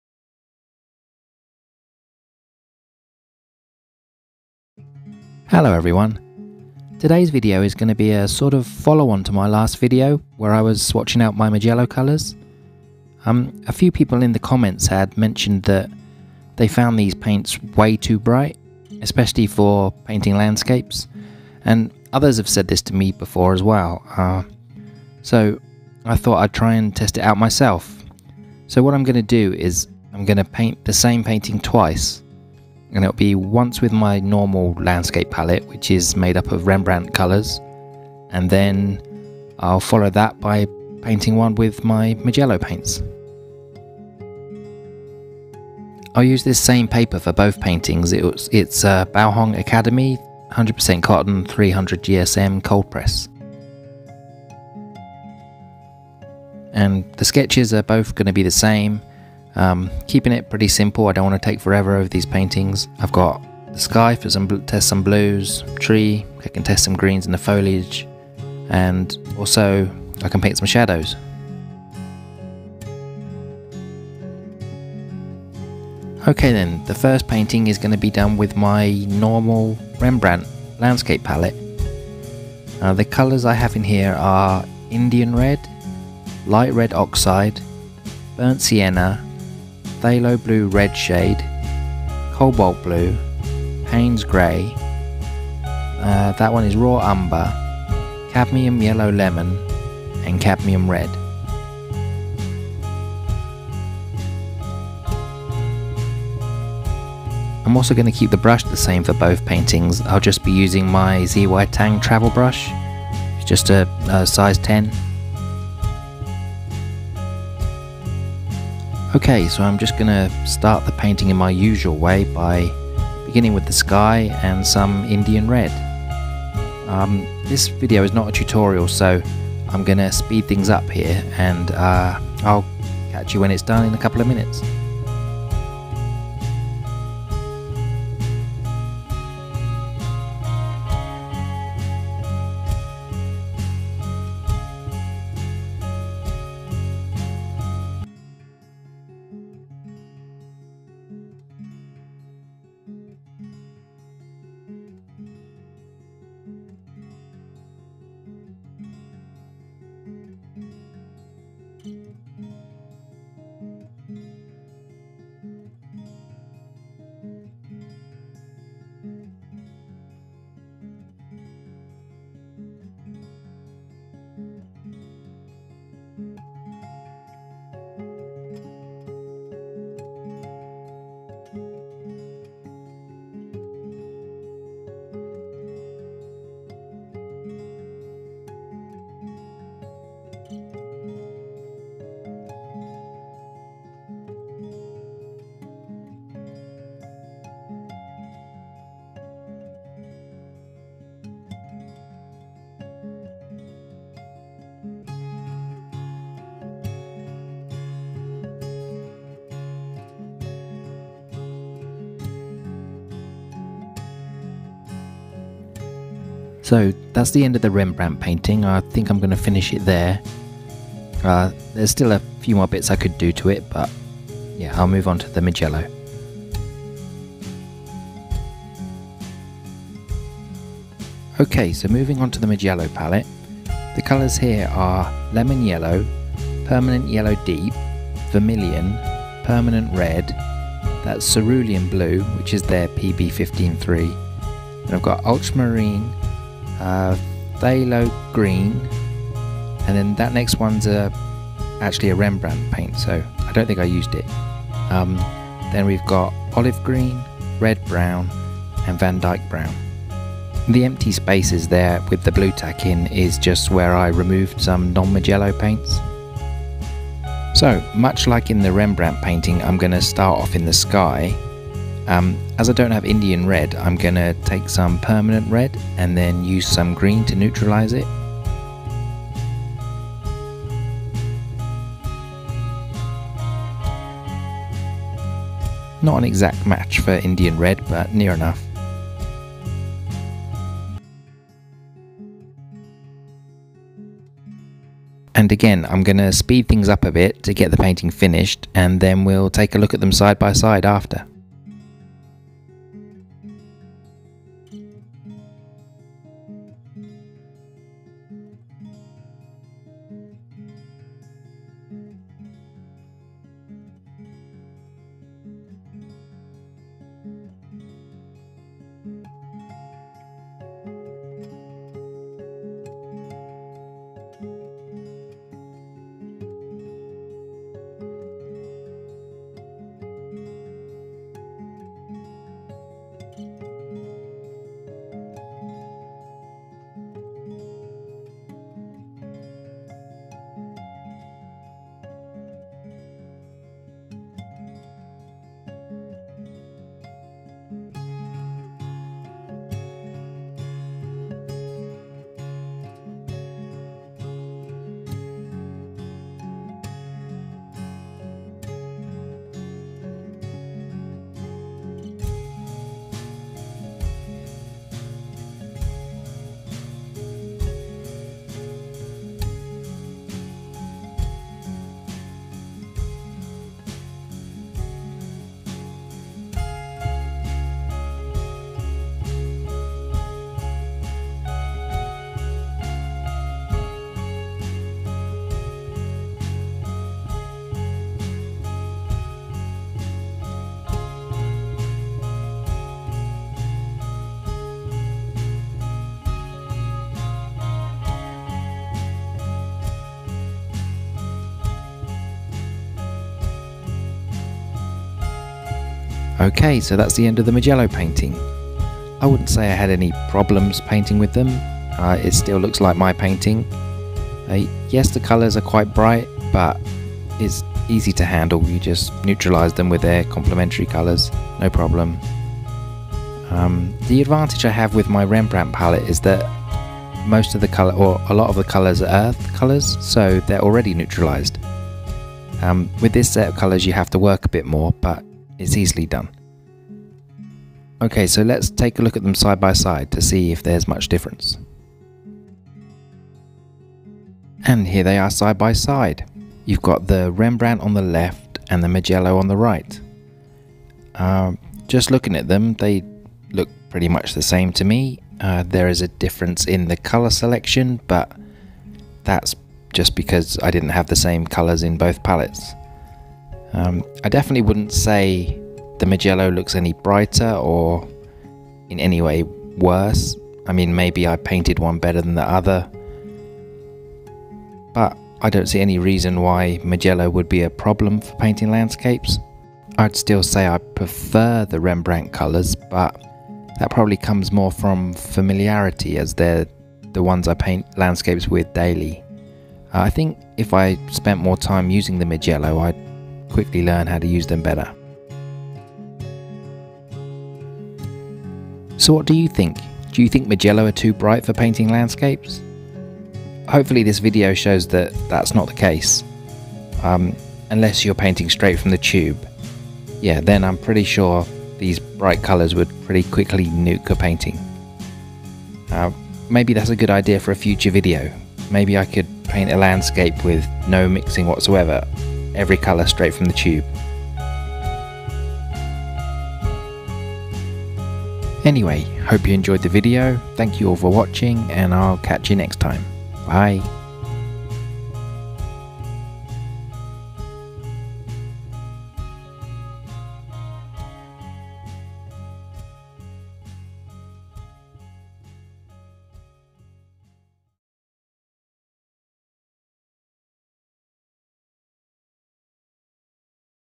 Are Mijello Mission Gold Watercolors Too Bright For Landscapes?
I have had a few people tell me, not only in the comment section of my last video but also before that, that Mijello Mission Gold Watercolors are too bright. Especially when it comes to painting landscapes.
In this video I wanted to put that to the test by painting the same painting twice. First with my usual Rembrandt Watercolors and then with my Mijello Mission Gold Watercolors.
Will the Mijello paints prove to be too bright? Or is it a simple case of neutralising them while mixing?

Zytang Travel Brush #10

Light Oxide Red
Old Version Unavailable

Raw Umber
Old Version Unavailable